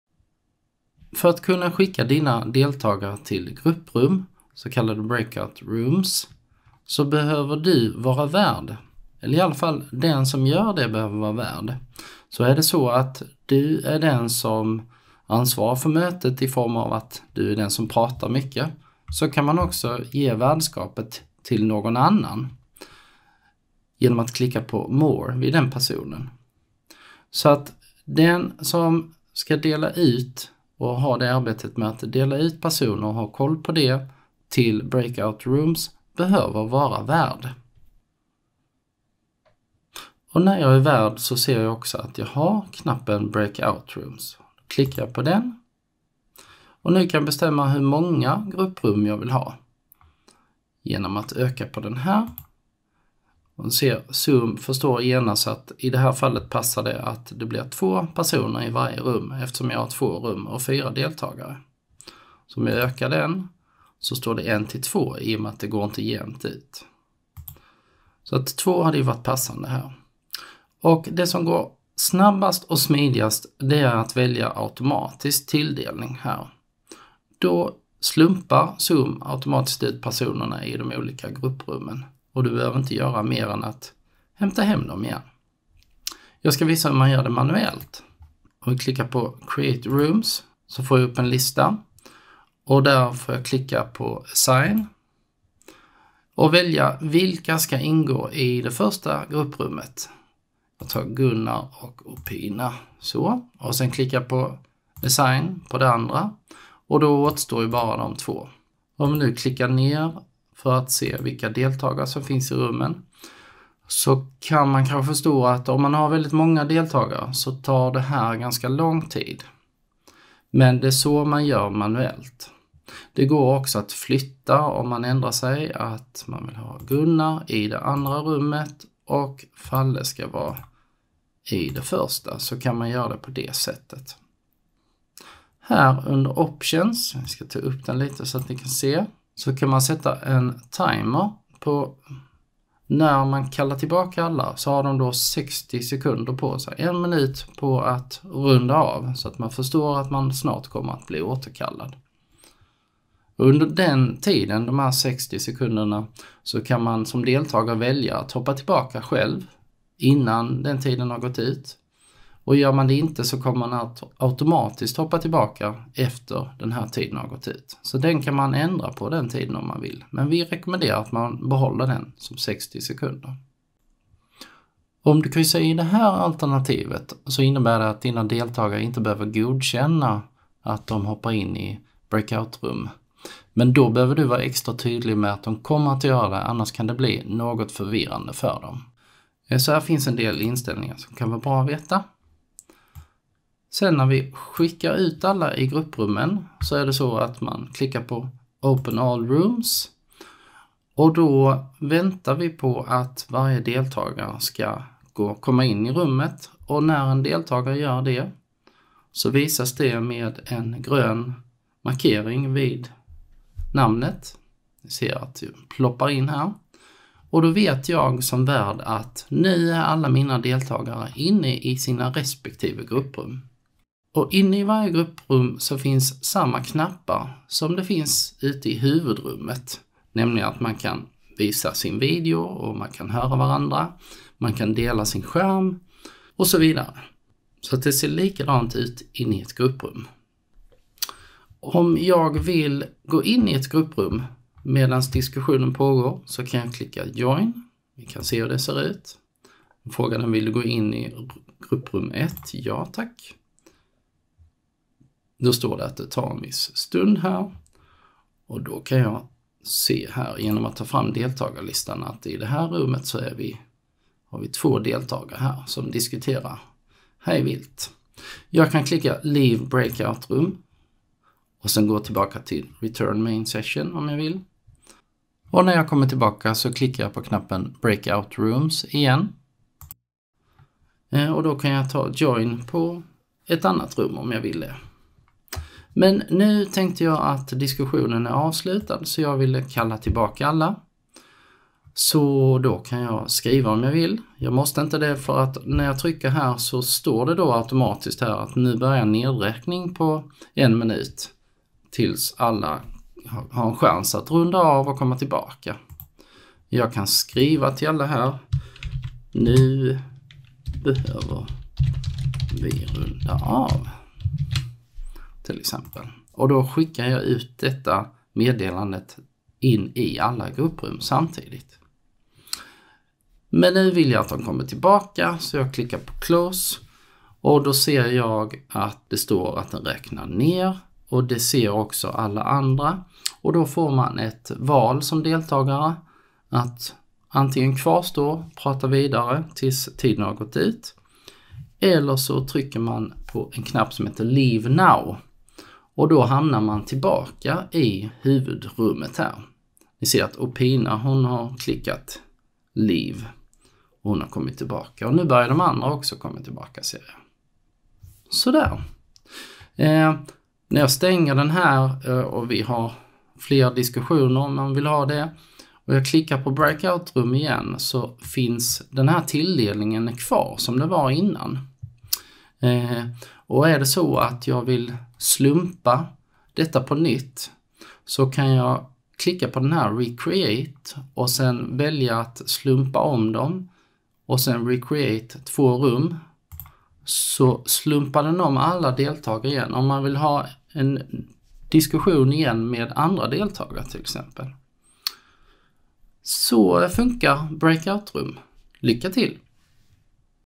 Zoom - hur Breakout Rooms fungerar
Lärresurs om grupprum i Zoom publicerad av Dan Wirdefalk, Bibliotek och högskolepedagogik, Högskolan Kristianstad.